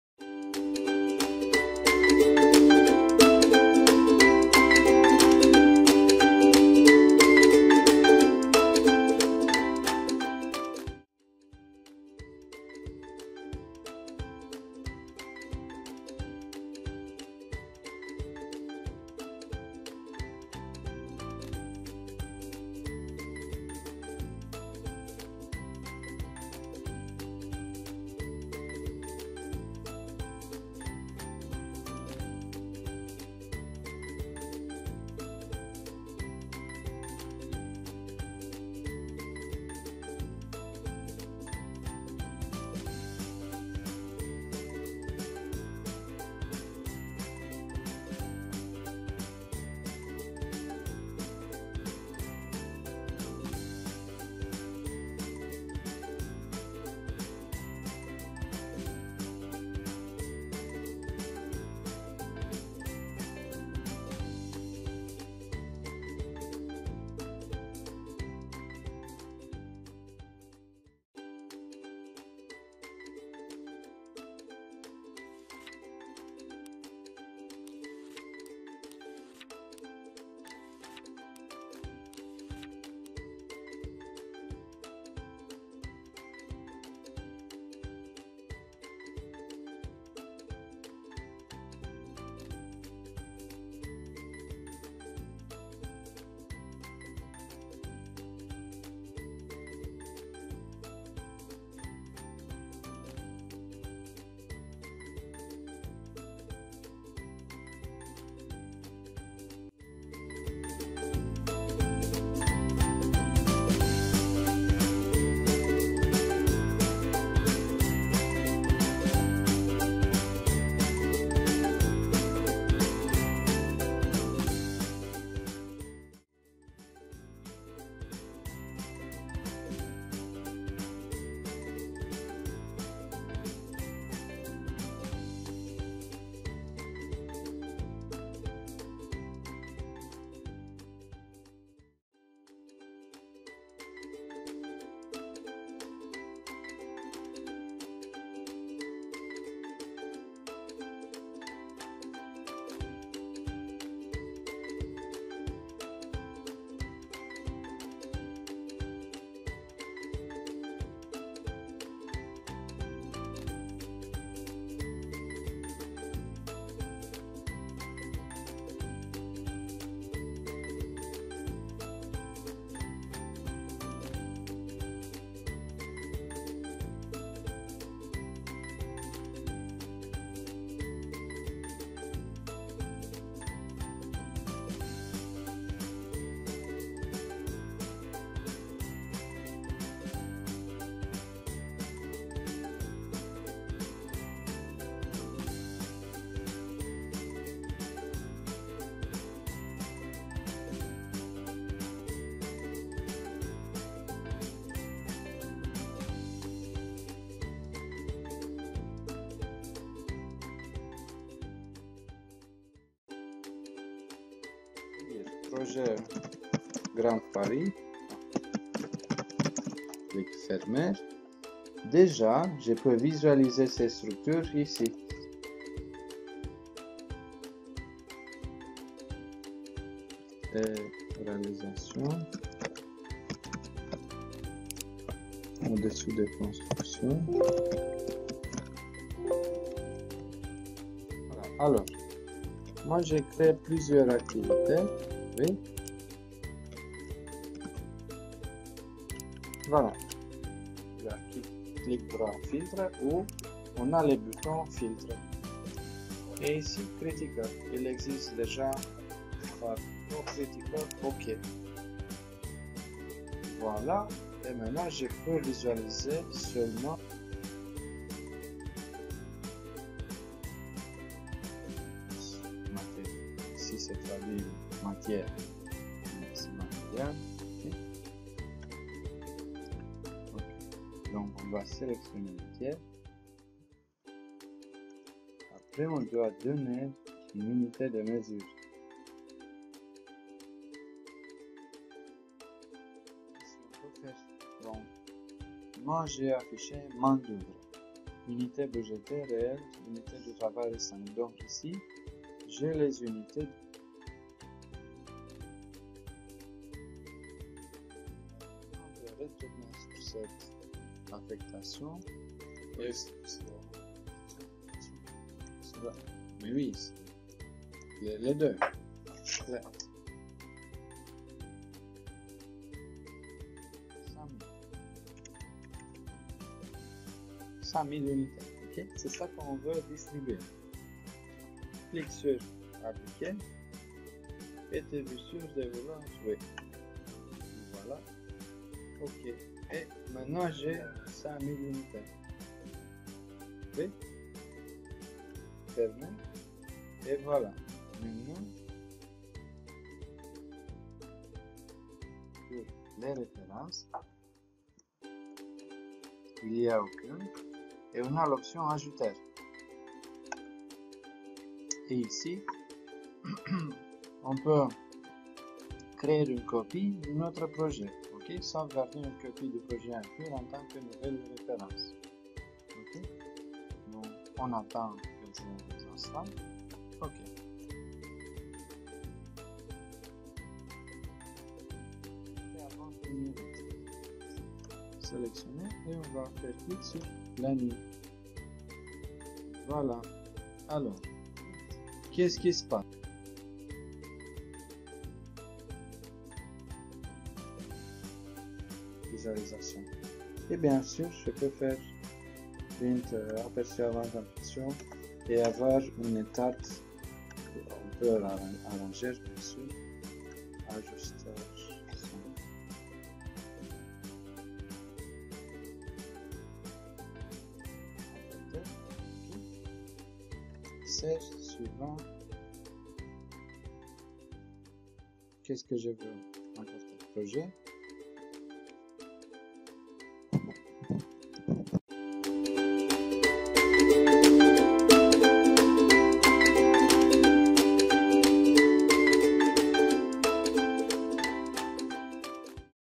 Formation Primavera: Planification et contrôle de projet avec Oracle Primavera®
Ce formation Primavera P6 vise à enseigner comment planifier et contrôler des projets en utilisant Oracle Primavera P6 18.8 et ses versions antérieures, dans le cadre des pratiques de gestion de projet définie par PMI (Project Management Institute).